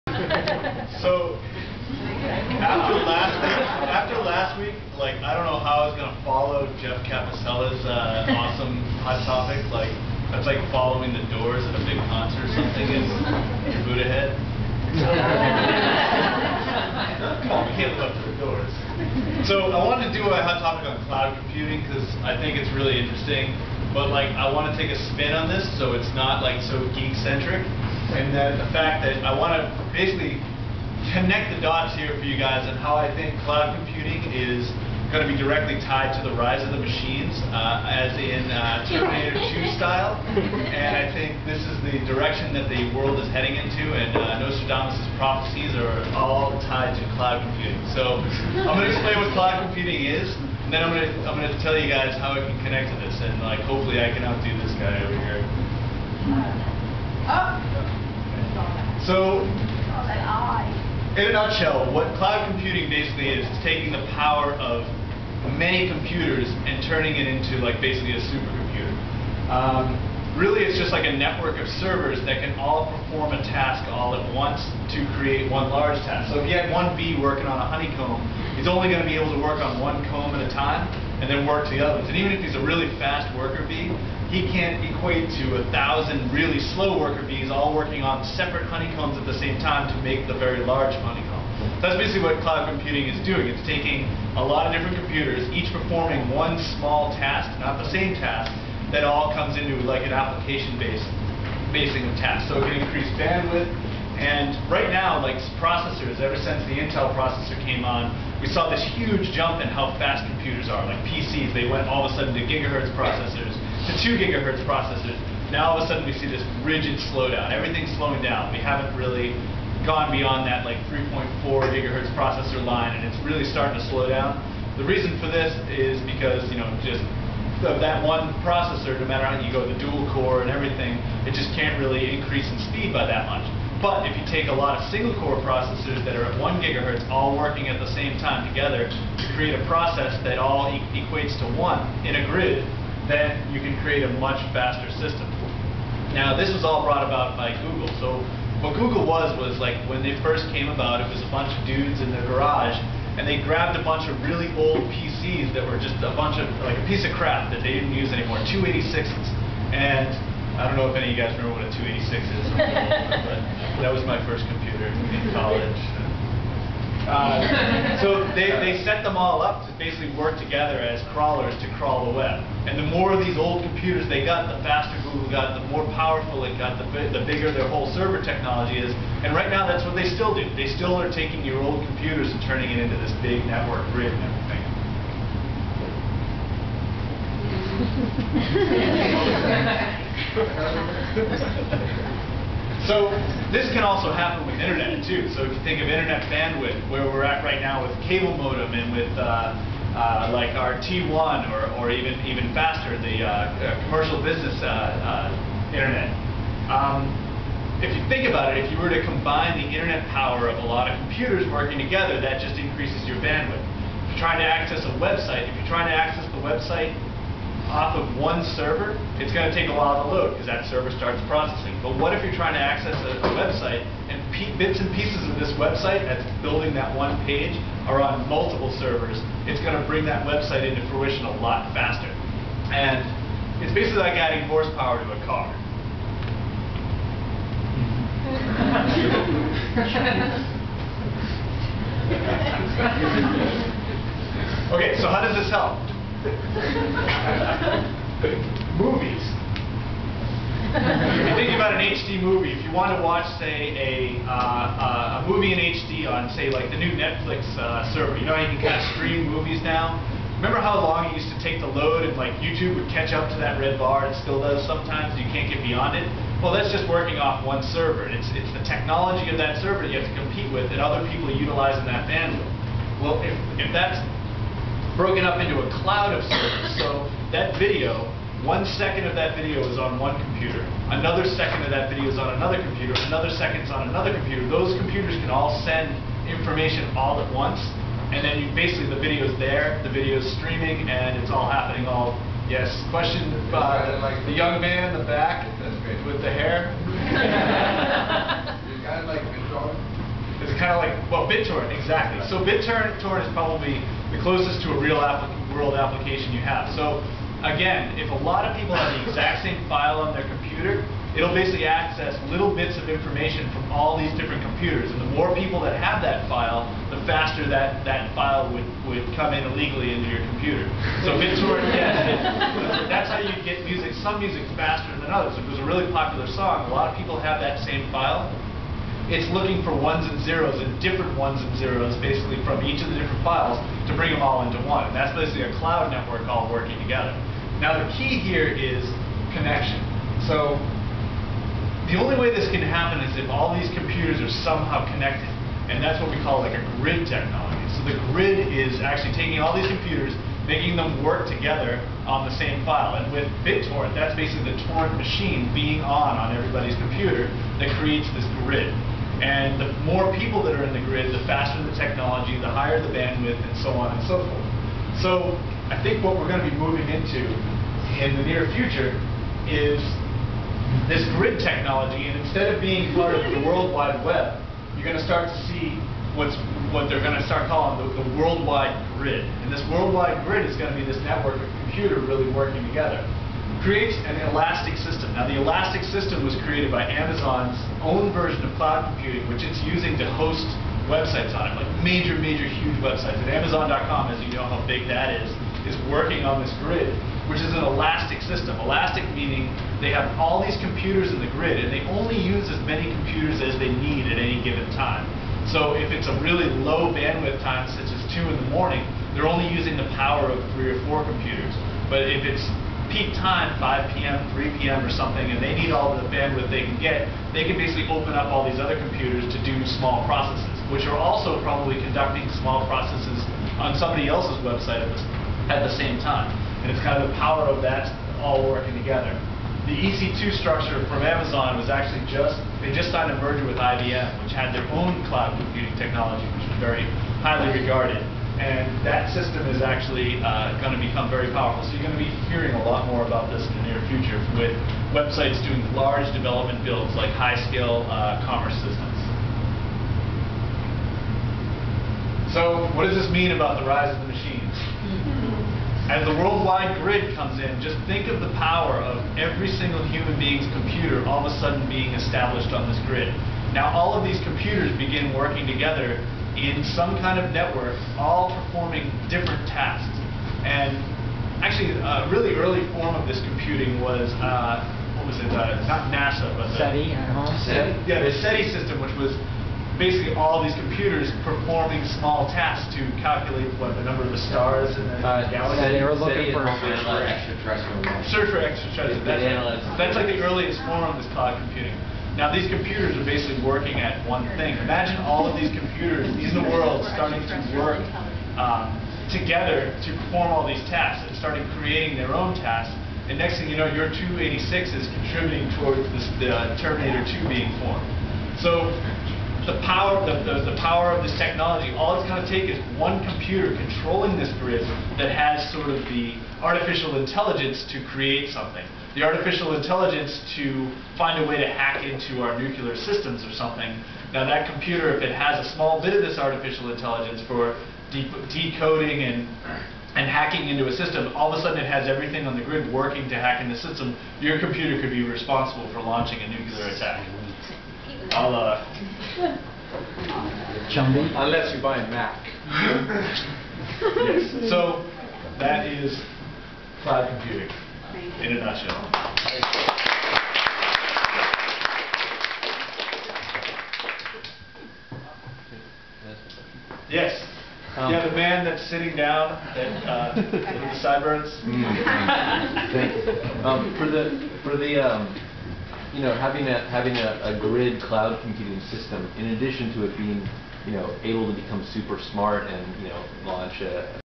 Toby Evers speaks about Cloud Computing
Toby Evers presenting his hot topic on Cloud Computing at Morpheus Media